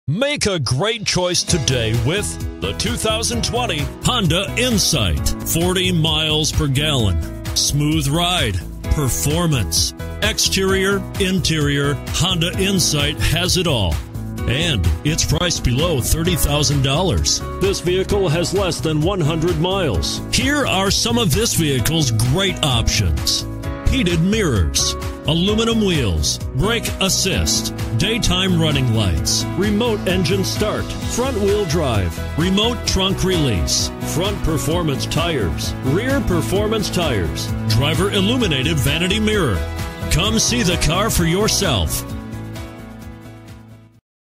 2020 Honda Insight 20HI4
2020 HONDA INSIGHT Tyler, TX
Stock# 20HI4

People can't stop talking about this highly touted 2020 Honda Insight EX.

* This Honda Insight EX Is Loaded with Features *
Wheels: 16  Aluminum Alloy, Wheels w/Machined w/Painted Accents Accents, VSA Electronic Stability Control (ESC), Variable Intermittent Wipers, Trunk Rear Cargo Access, Trip Computer, Transmission: Electric Continuously Variable -inc: ECON, Sport and EV drive modes, Shift-By-Wire (SBW) and deceleration selectors, Tires: 215/55R16 93V AS, Tailgate/Rear Door Lock Included w/Power Door Locks, Systems Monitor.